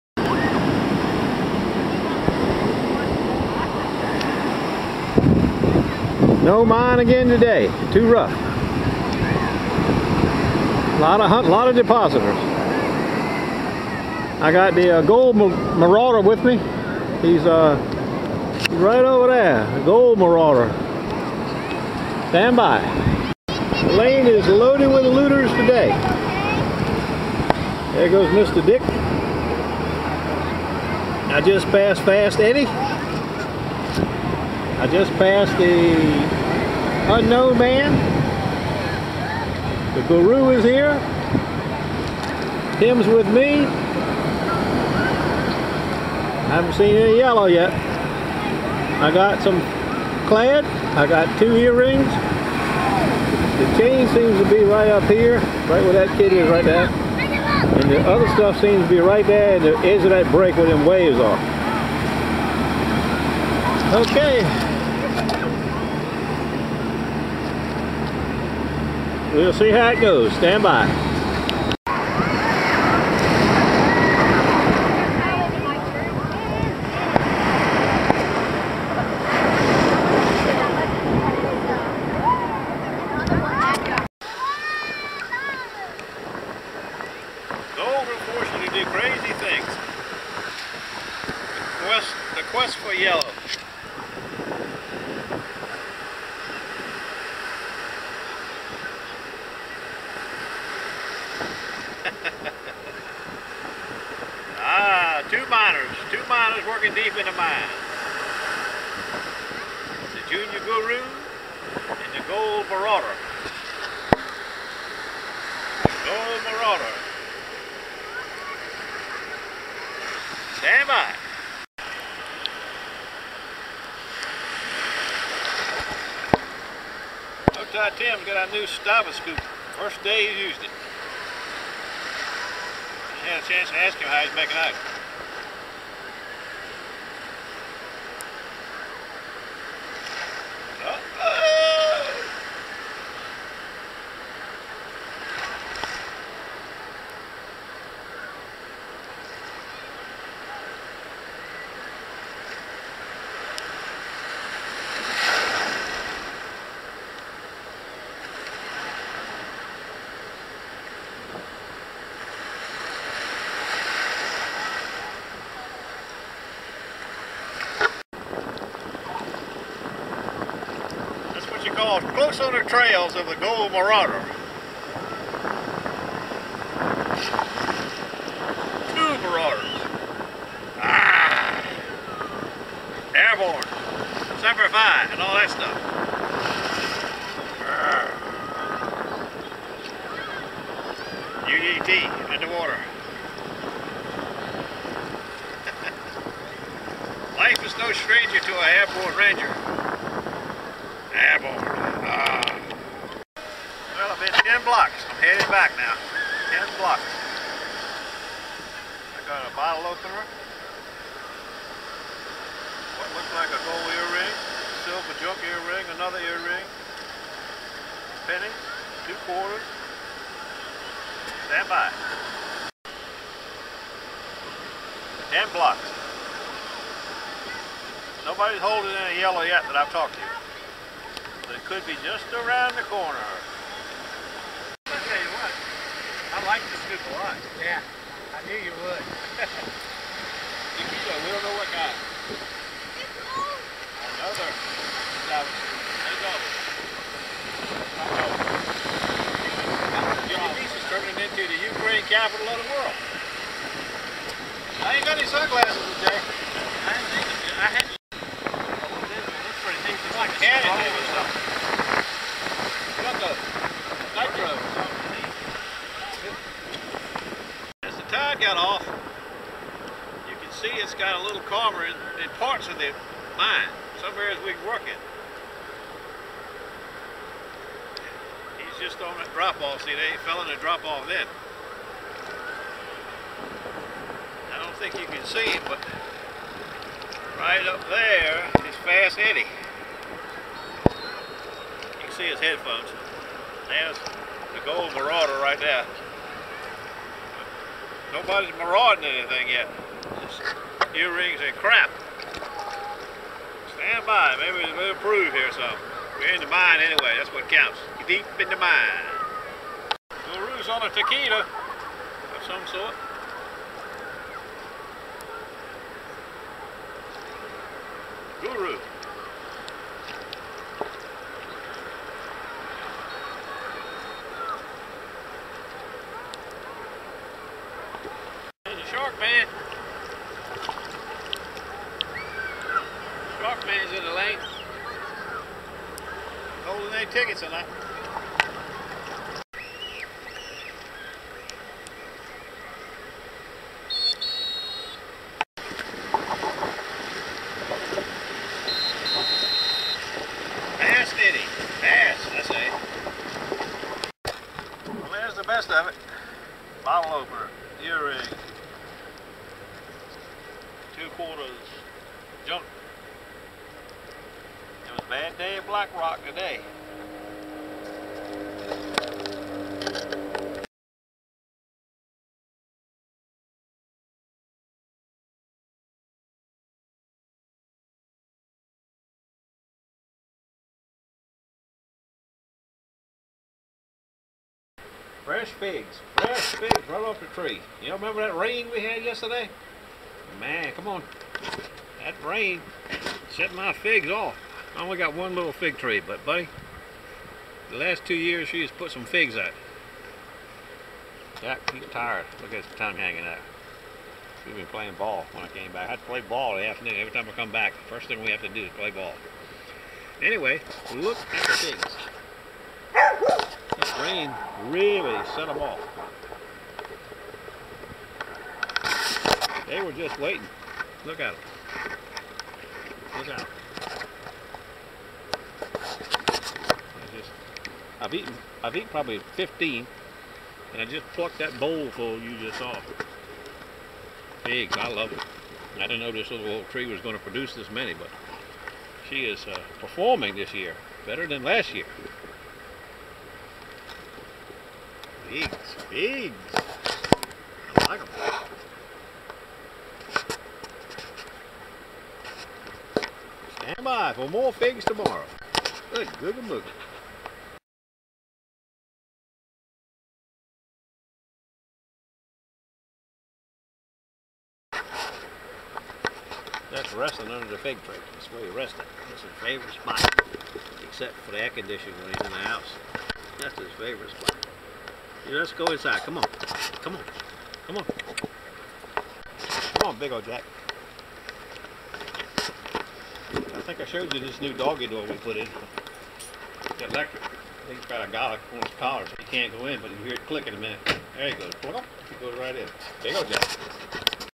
JULY 22ND - Surfchurch with the Gold Maurader - bad day a black rock today
Surfchurch was loaded with depositors and looters, but the yellow was not to be seen or at our leaving it was still missing.
One fellow -- Dice did have two rings -- one looked ok, but jury is still out on it
The gold marauder had a bracelet and some earrings like most of us today, but he did say he really loved his new scoop.
The weather held and we got to hunt our normal 4 hours today and waves decreased and the mine was accessible in certain areas, but no gold veins to be found.
On a side note:  called ken to see how he made out and he said zip, but he wanted to know if we saw him step into that hole -- I said no, apparently soon after I shot the vid of him and tim hunting close by, he stepped in a deep hole and went under water.  He said that was ok, but about 10mins. later he is digging a target in waist deep zone and big wave hit him and knocked him flat on his face and washed him up the beach  -  has happened to me many times and will happen again as well -- "the lane"
See you Monday  - good luck out there -